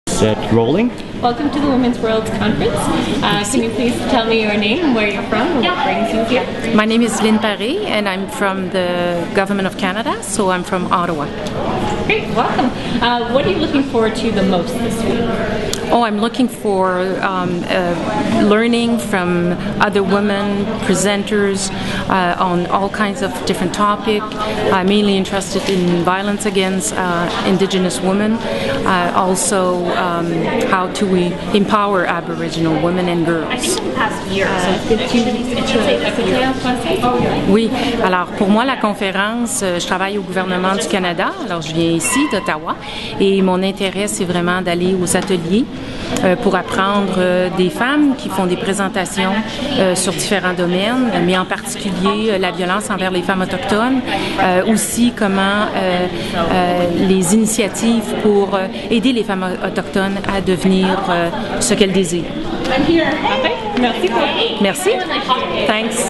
Lynne Paré @ Indigenous Feminisms ROCK! -- Women's Worlds 2011
At the Indigenous Feminisms ROCK! event on 4 July at Women's Worlds 2011, Lynne Paré, an employee of the Canadian federal government, discusses her interest in issues affecting Aboriginal women.